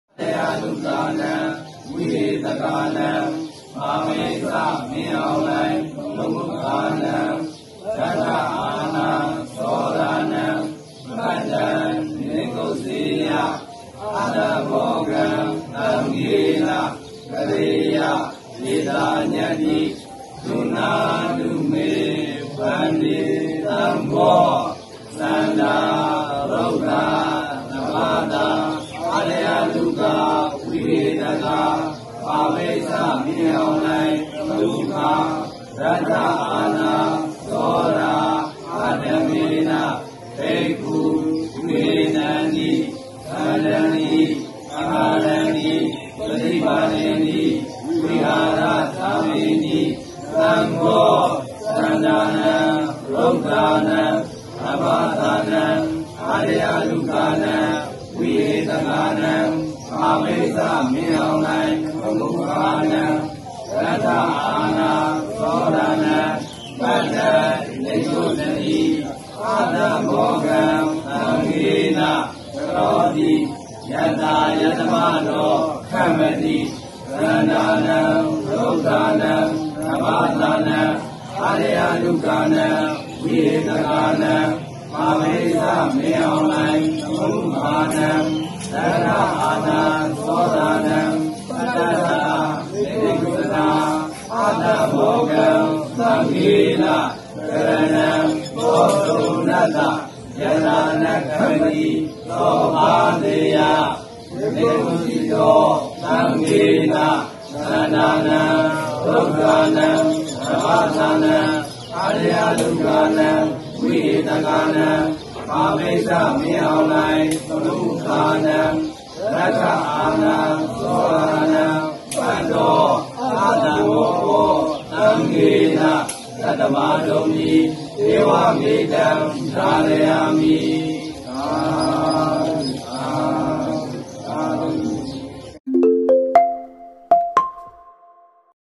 မင်းအောင်လှိုင် နာမည်ကို ထည့်သွင်းပြီး စစ်ကိုင်းရှိသံဃာတော်တွေက ပတ္တနိက္ကုဇ္ဇန ကံဆောင်၍ ကမ္မဝါစာရွတ်
◼ စစ်ကိုင်းဒေသ ချောင်းဦးမြို့နယ်ရှိ သံဃာတော်တွေက အကြမ်းဖက်စစ်ကောင်စီကို  ဇွန် ၂၃ ရက်မှာ ပတ္တနိက္ကုဇ္ဇန ကံဆောင်၍ ကမ္မဝါစာရွတ်ဖတ်နေတဲ့ ရုပ်သံမှတ်တမ်းကို ကြည့်ရှုရမှာ ဖြစ်ပါတယ်။